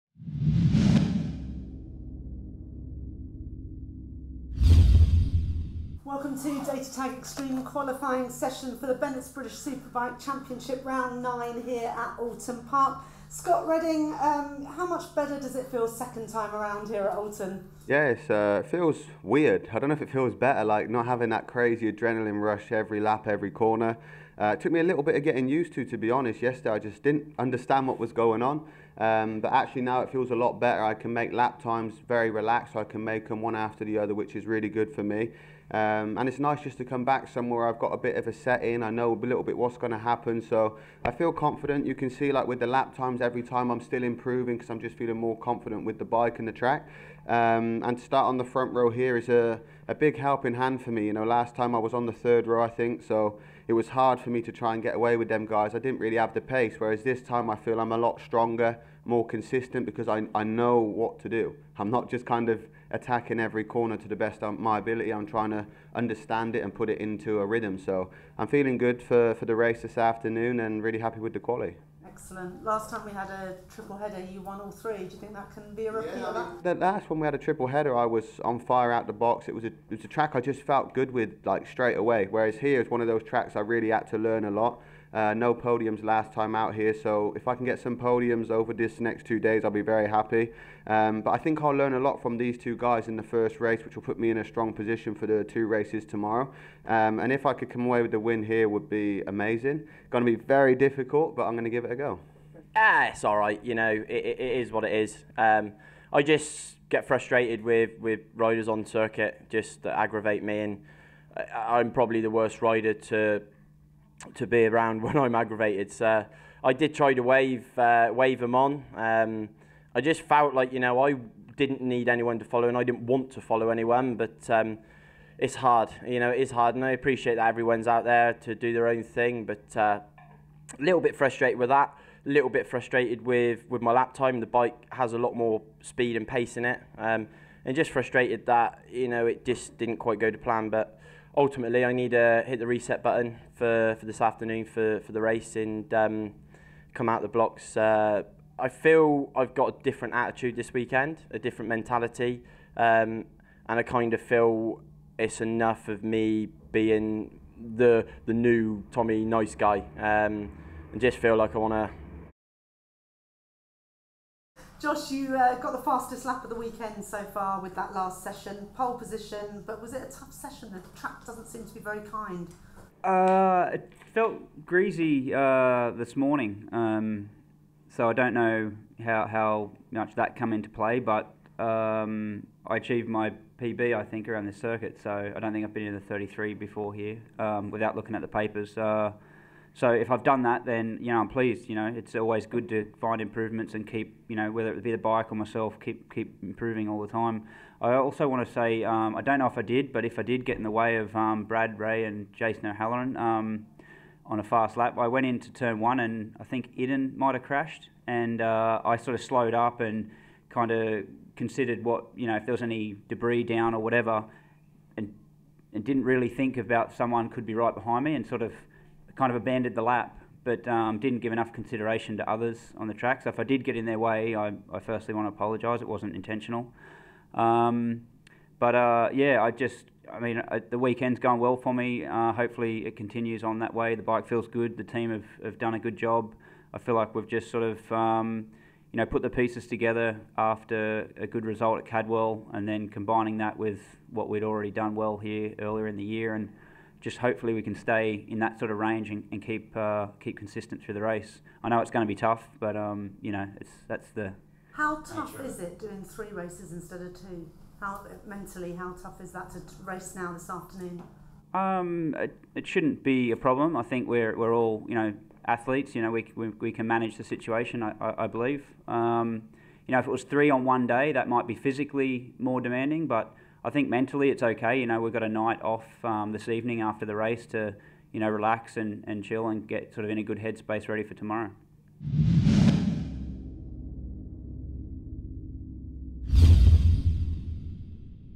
2019 Round 9 - Oulton Park Datatag Qualifying press conference
The front row riders in Bennetts BSB Datatag Qualifying from Oulton Park reflect on their performances ahead of race one this afternoon